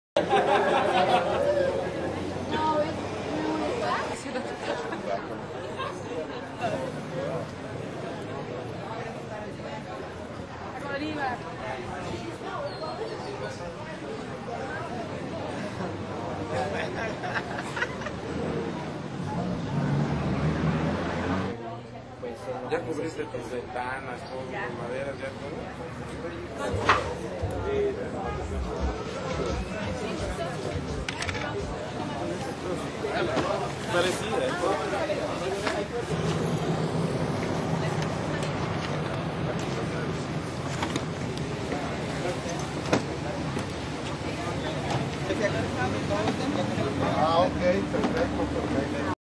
Tulum - Mexico
Mexico prepares for the hurricane Dean